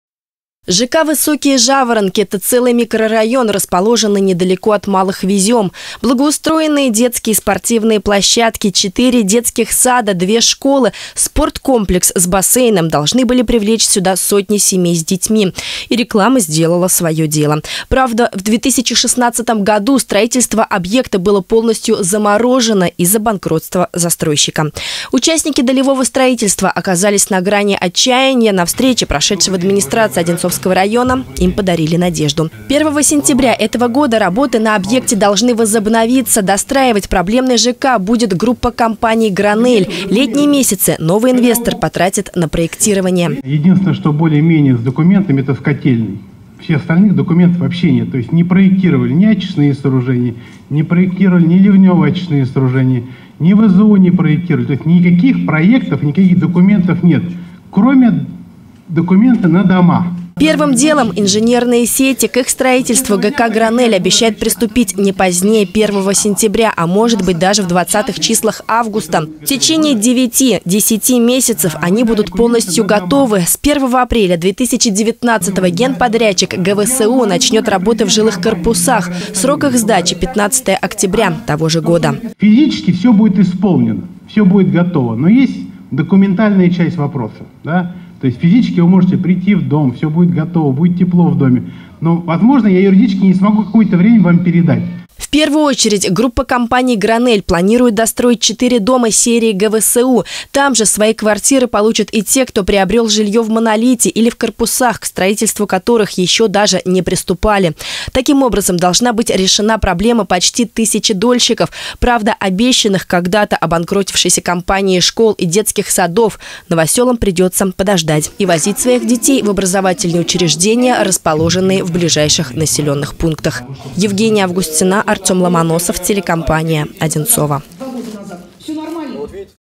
Новый инвестор ЖК «Высокие Жаворонки» приступил к работе
В Одинцово прошла встреча участников долевого строительства с новым инвестором ЖК «Высокие Жаворонки», который уже приступил к работам на недостроенном объекте.